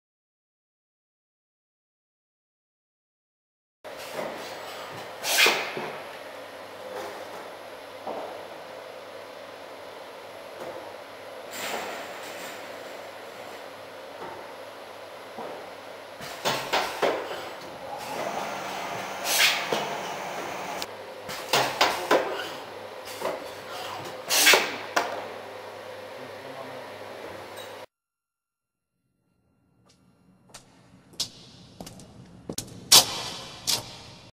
GDK blowmoulding machine GM251
Fully electrical single station blow moulding machine for the production of plastic bottles and containers up to 2,5 liters